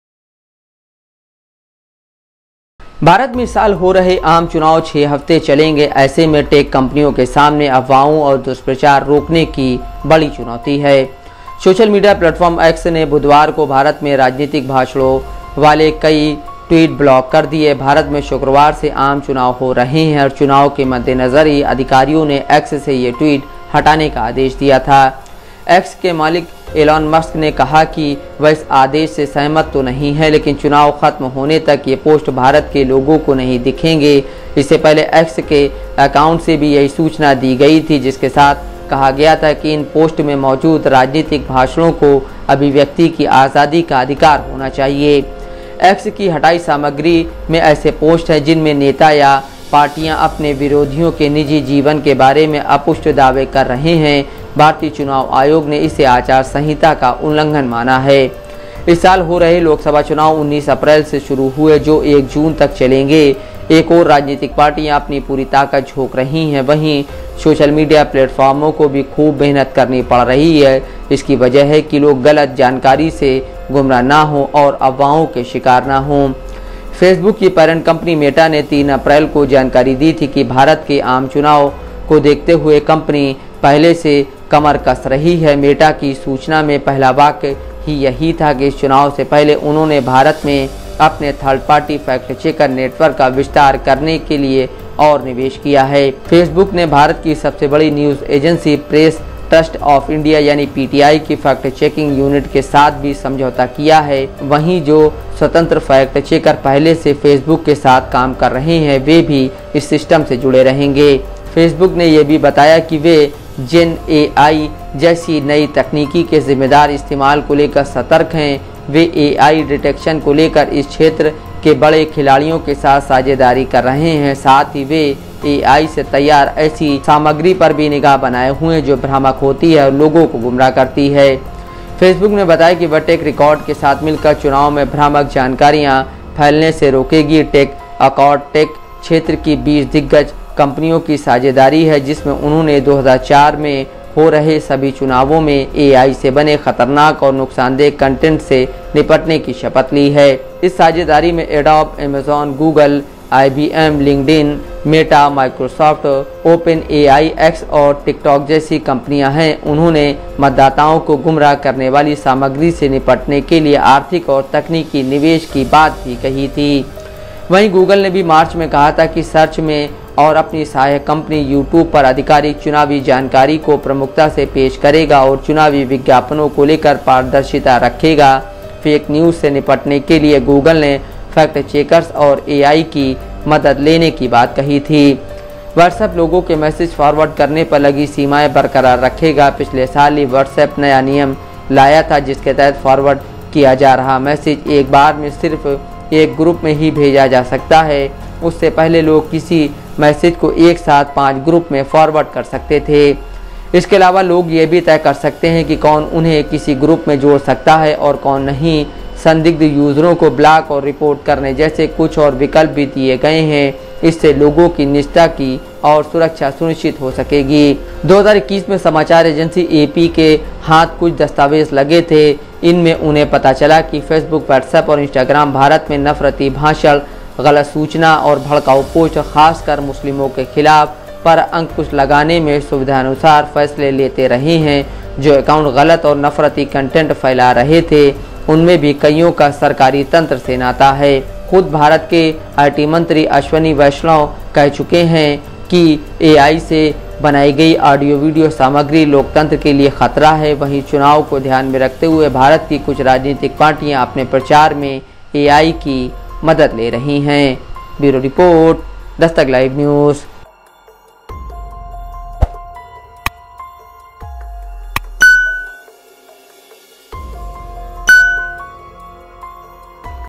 Lok Sabha Election 2024 : Bharat मे ईमानदार Election के लिए Tech कंपनिया क्या कर रही है‚
भारत में इस साल हो रहे आम चुनाव 6 हफ्ते चलेंगे. ऐसे में टेक कंपनियों के सामने अफवाहों और दुष्प्रचार रोकने की बड़ी चुनौती है।

सोशल मीडिया प्लेटफॉर्म एक्स ने बुधवार को भारत में राजनीतिक भाषणों वालों कई ट्वीट ब्लॉक कर दिए. भारत में शुक्रवार से आम चुनाव हो रहे हैं और चुनाव के मद्देनजर ही अधिकारियों ने एक्स से ये ट्वीट हटाने का आदेश दिया था.

एक्स के मालिक इलॉन मस्क ने कहा कि वह इस आदेश से सहमत तो नहीं हैं, लेकिन चुनाव खत्म होने तक ये पोस्ट भारत के लोगों को नहीं दिखेंगे. इससे पहले एक्स के अकाउंट से भी यही सूचना दी गई थी, जिसके साथ कहा गया था कि इन पोस्ट में मौजूद राजनीतिक भाषणों को अभिव्यक्ति की आजादी का अधिकार होना चाहिए.

एक्स की हटाई सामग्री में ऐसे पोस्ट हैं, जिनमें नेता या पार्टियां अपने विरोधियों के निजी जीवन के बारे में अपुष्ट दावे कर रहे हैं. भारतीय चुनाव आयोग ने इसे आचार संहिता का उल्लंधन माना है.

इस साल हो रहे लोकसभा चुनाव 19 अप्रैल से शुरू हुए जो 1 जून तक चलेंगे. एक ओर राजनीतिक पार्टियां अपनी पूरी ताकत झोंक रही हैं. वहीं सोशल मीडिया प्लेटफॉर्मों को भी खूब मेहनत करनी पड़ रही है. इसकी वजह है कि लोग गलत जानकारी से गुमराह ना हों और अफवाहों के शिकार ना बनें.